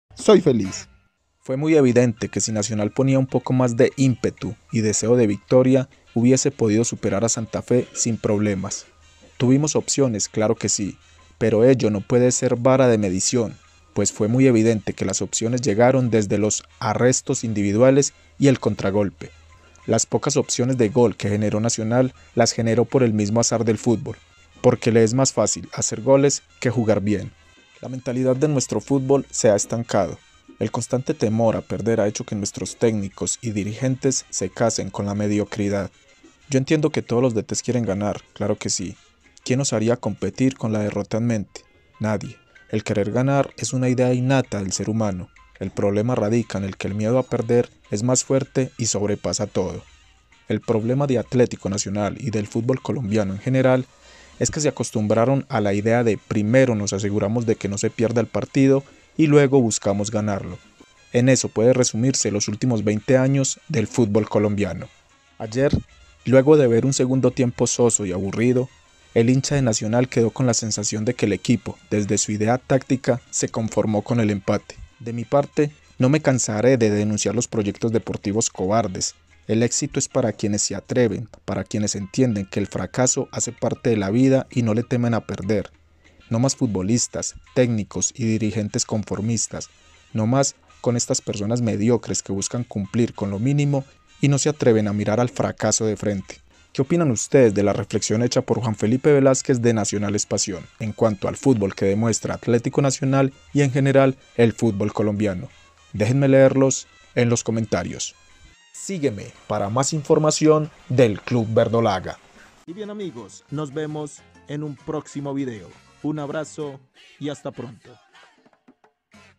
🔥FUERTES PALABRAS🤔 ¡FUTBOLISTAS, TÉCNICOS Y DIRIGENTES CONFORMISTAS Y MIEDOSOS/ NO MÁS🔥
En la retina del hincha verdolaga queda la impresión de que en Atlético Nacional se estan conformando con resultafos mediocres y que tanto jugadores como cuerpo tecnico y directivos se estan acostumbrando a un futbol miedoso y de pobres resultados. 🤔